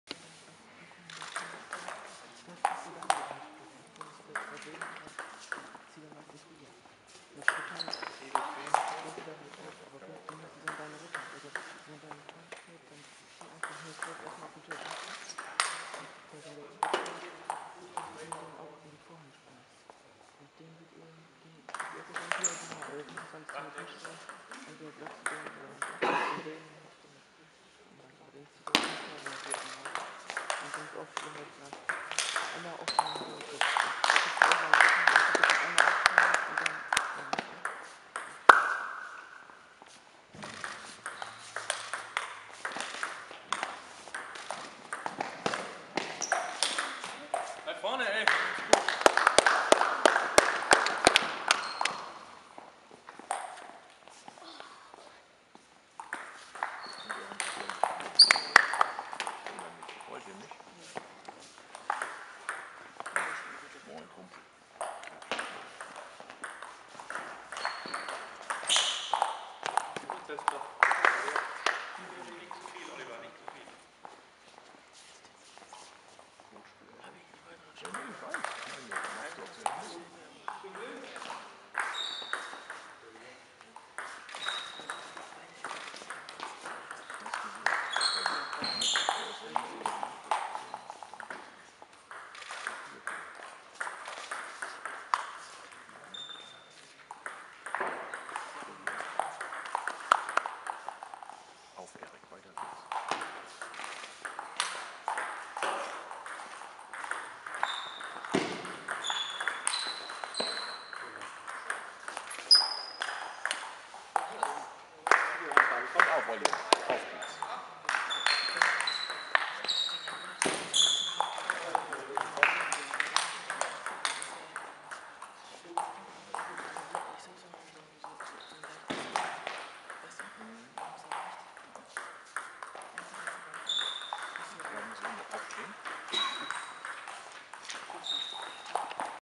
Jürgens vs. Eggers
Punktspiel der 1. Jungen in der Niedersachsenliga 2017 gegen den TTC Lechstedt (Endstand 3:1)